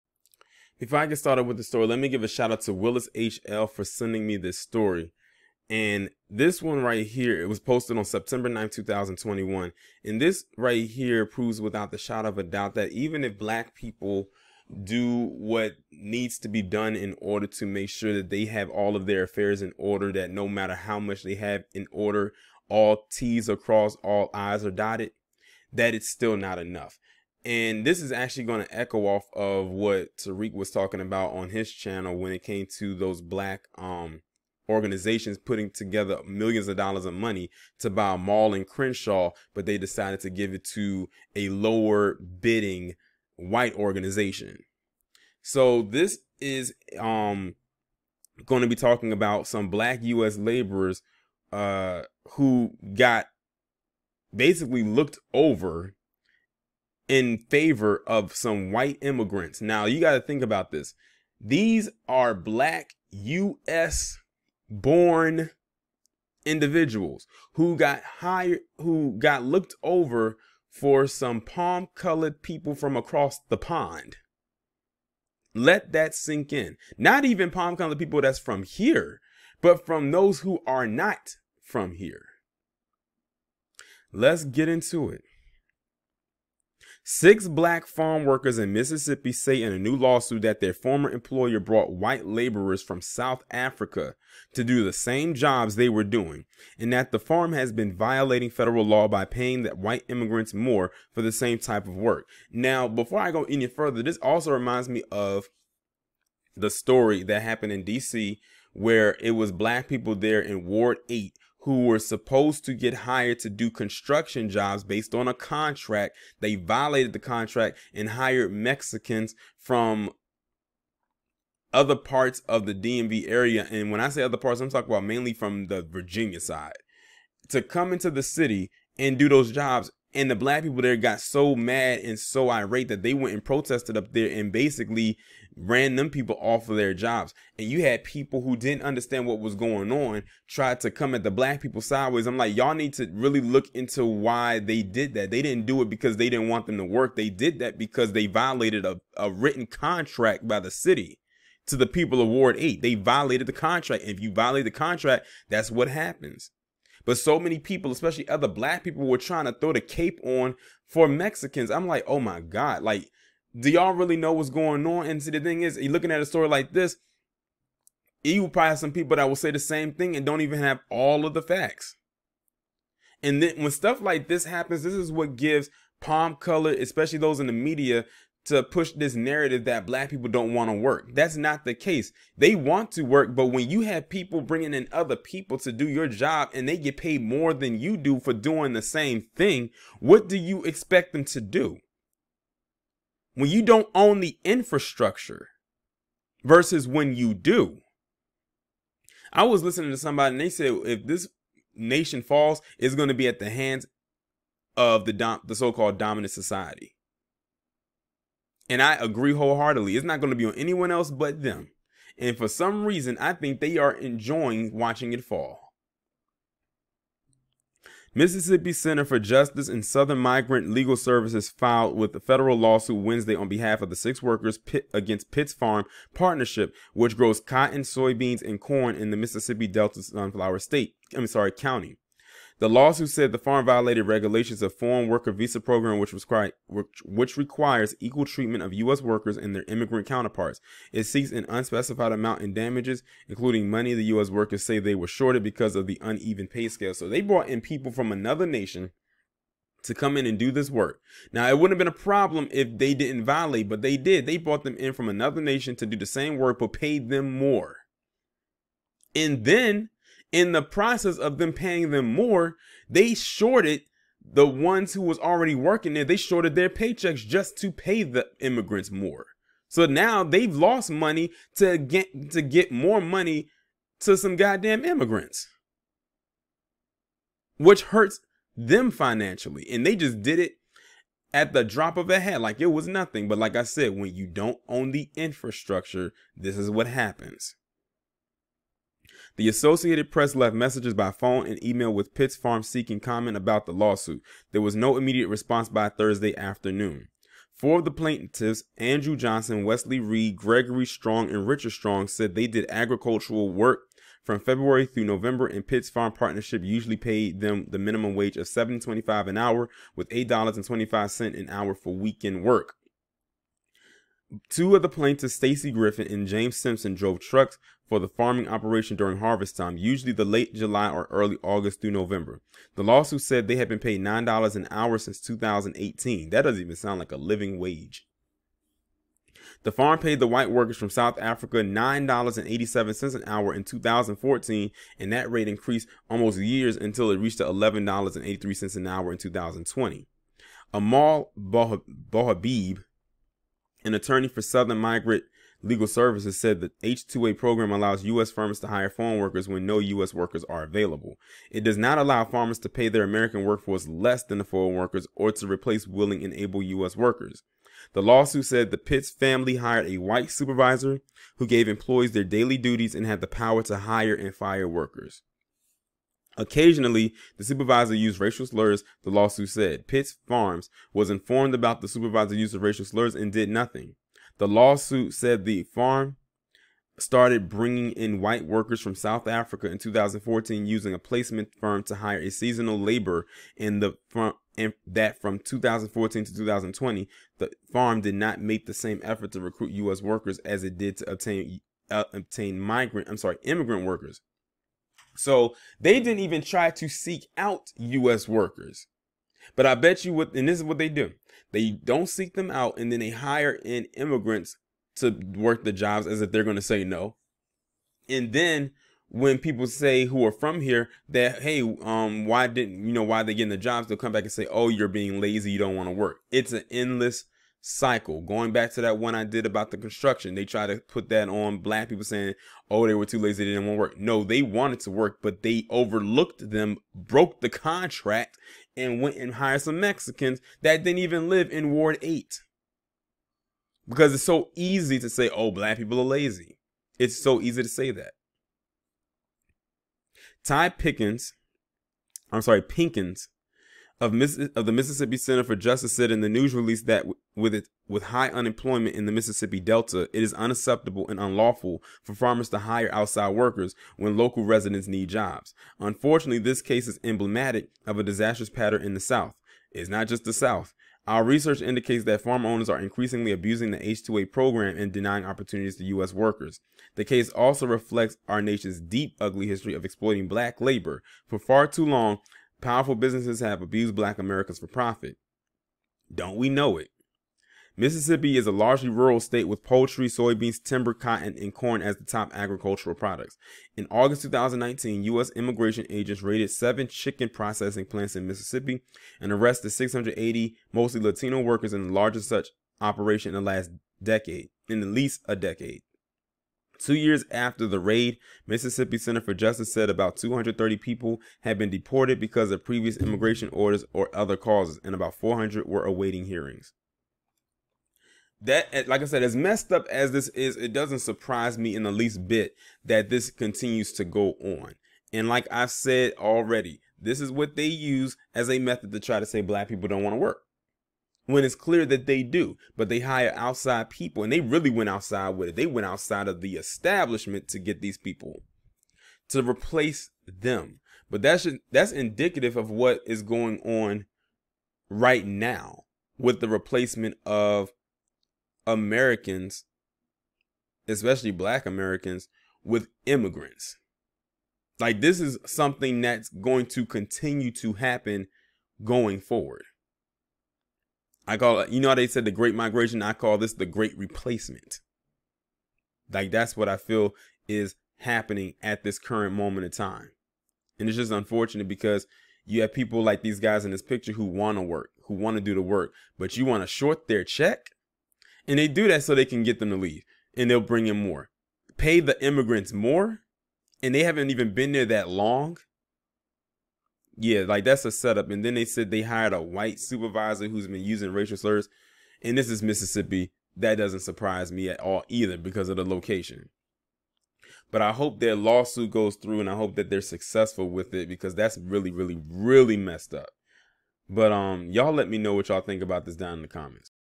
The Great Replacement
Black farmers in the US are facing the harsh reality of being replaced from outsiders (mainly white immigrants) while being shut out from resources needed to continue the growth of their farming business.